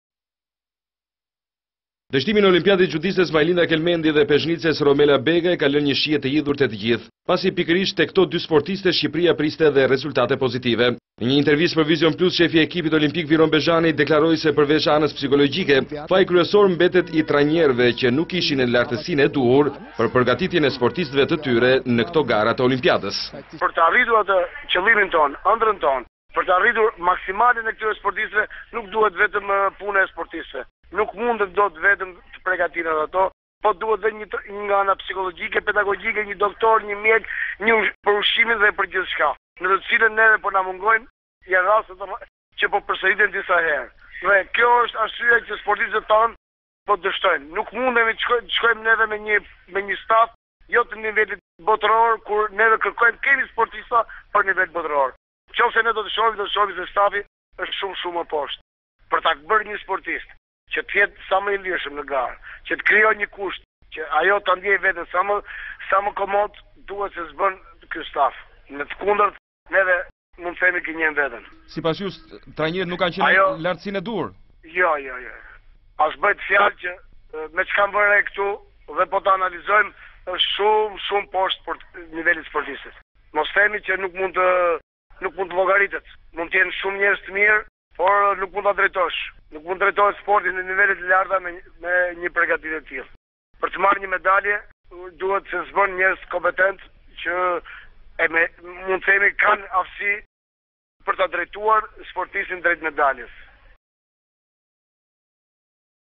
Bezhani: Dështimi është i trajnerëve - Vizion Plus - News - Lajme
Dështimi ne olimpiade i xhudistes Majlinda Kelmendi dhe peshëngritëses Romela Begaj ka lënë një shije te hidhur tek te gjithë, pasi pikërisht te këto dy sportiste Shqipëria priste dhe rezultatet pozitive. Ndërkohë, ne një interviste për "Vizion Plus", shefi i ekipit olimpik, Viron Bezhani, deklaroi se përveç anës psikologjike, faji kryesor mbetet i trajnerëve te sportisteve, qe nuk ishin ne lartësinë e duhur për përgatitjen ne këto gara te olimpiadës.

Failure of xhudistes Olympics Majlinda Kelmendi Romela peshëngritëses Begaj has left a bitter taste at all, because precisely these two athletes and Albania expect positive results. Meanwhile, in an interview for "Vision Plus" Olympic team boss, Viron Bezhani, said that apart from the psychological side, the main fault remains coaches of athletes, who were not at the level of preparation for the Olympiad competitions.